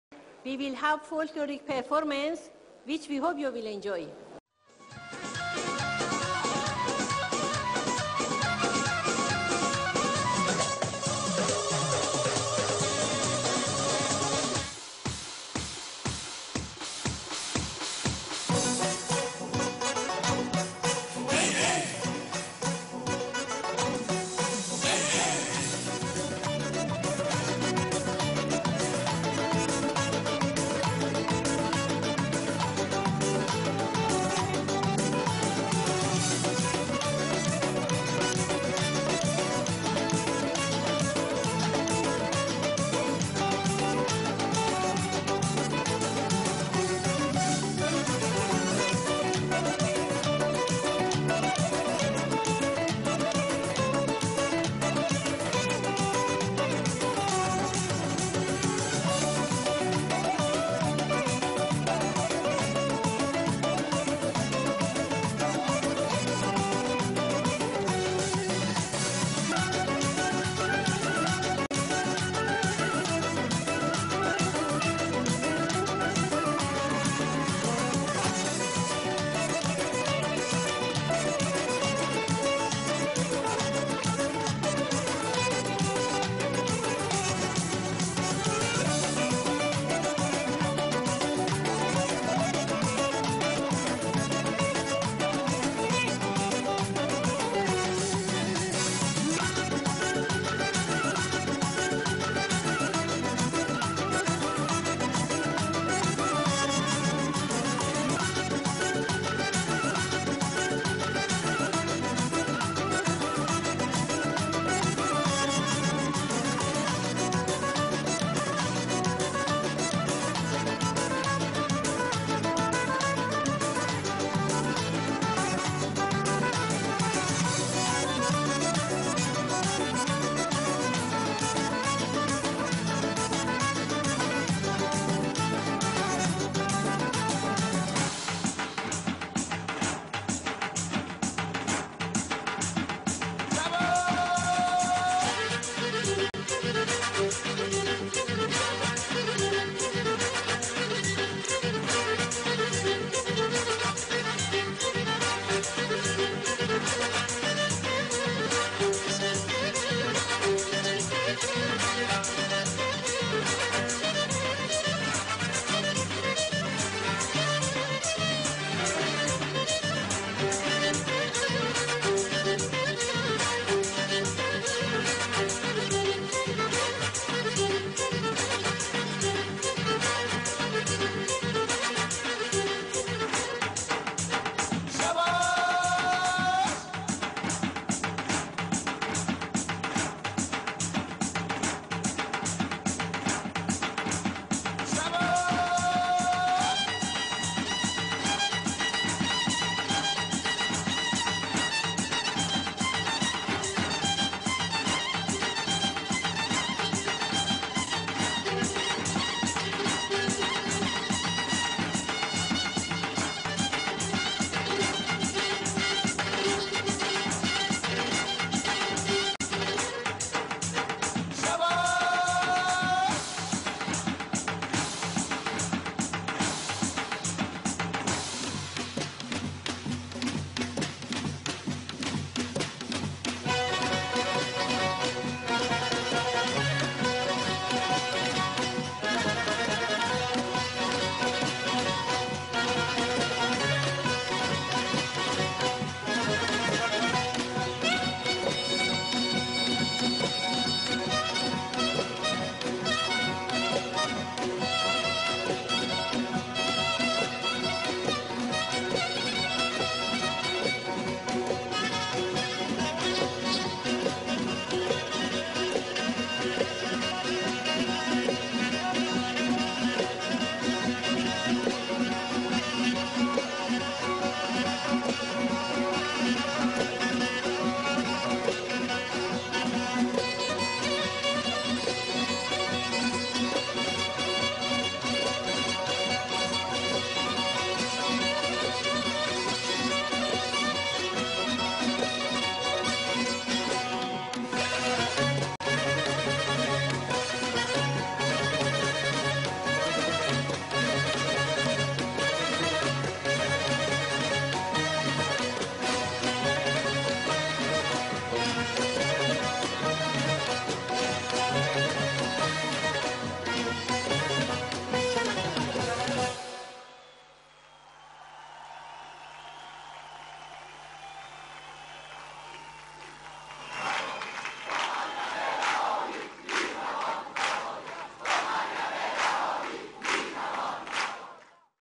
Iranian folkloric dance at Nowruz celebrations with Iranian Resistance in Albania
Iranian folkloric dance at Nowruz celebrations with Iranian Resistance members and supporters in Albania